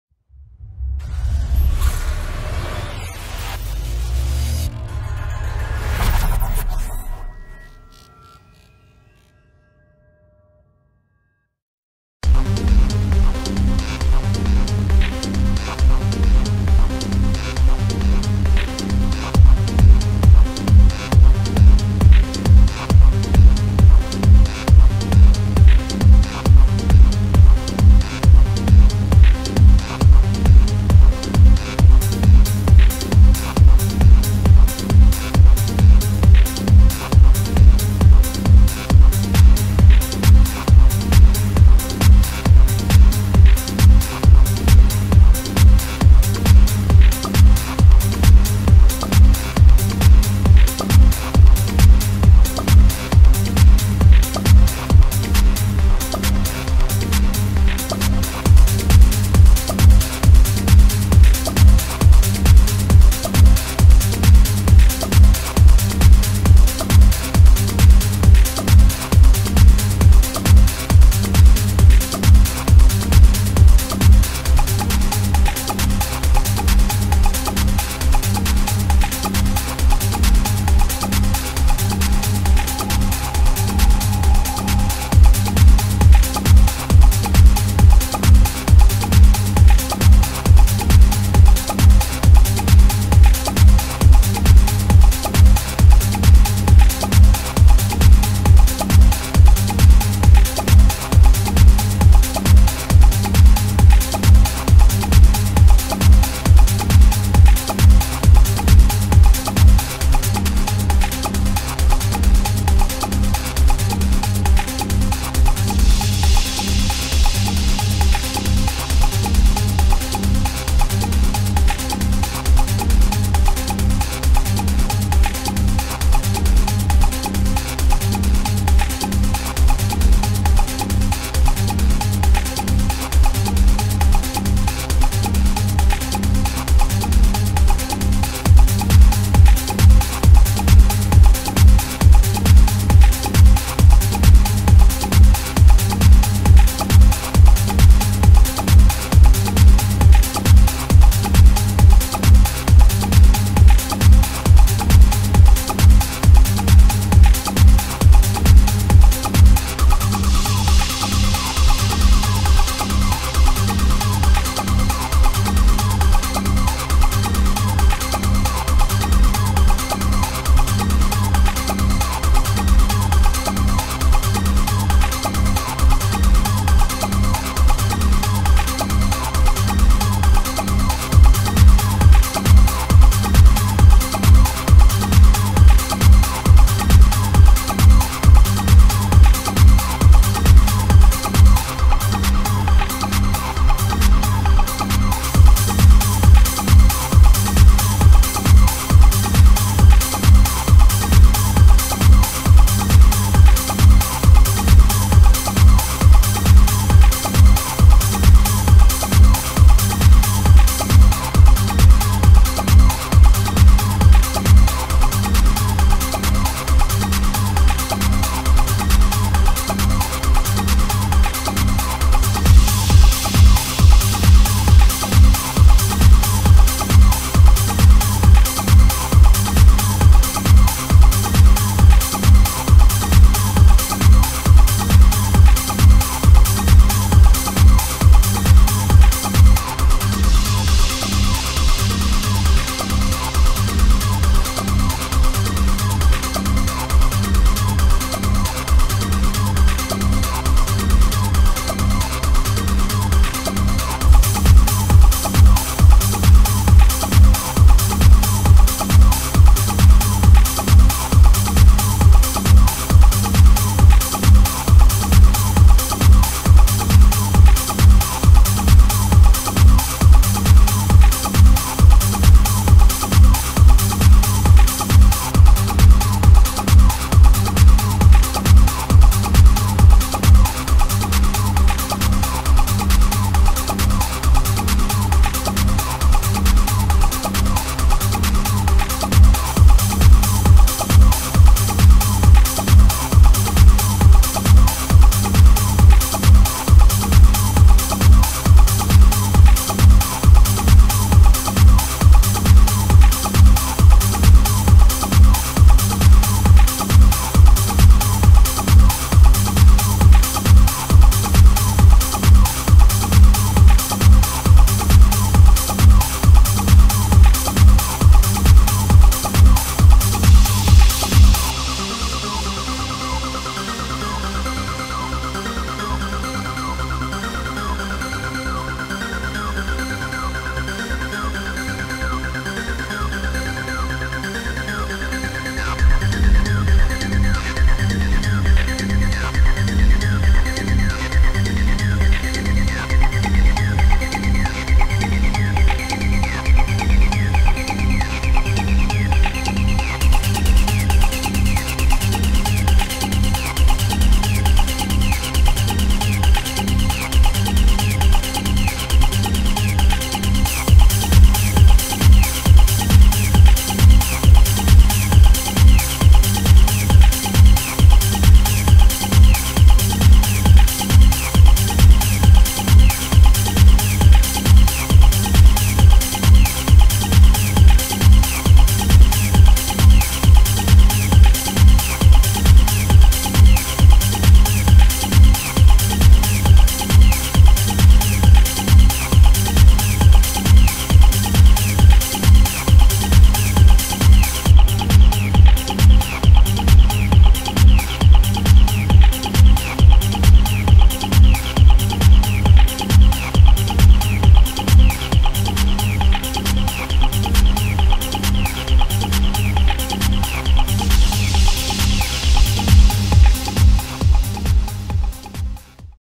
From Electro to Techno - Rock Center
"From Electro to Techno"

Veranstaltung im Rock Center in Bremerhaven am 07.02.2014

Videomusik: DJ Adrian Bäcker
Filmproduktion: Stephan Brannys